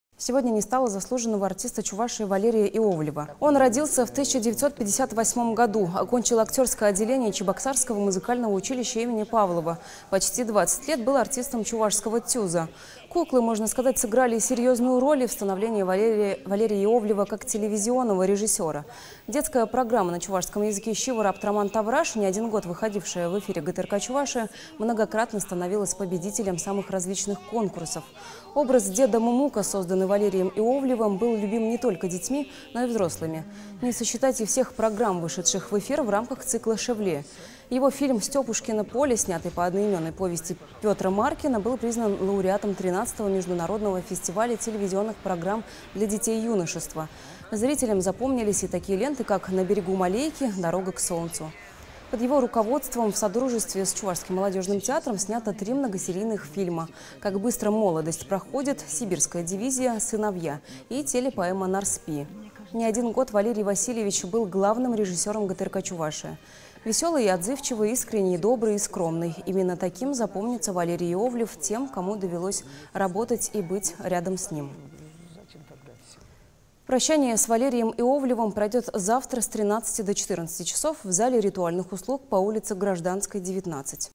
Ушел из жизни заслуженный артист Чувашии Валерий Иовлев
Сегодня не стало заслуженного артиста Чувашии Валерия Иовлева.

Он родился в 1958 году.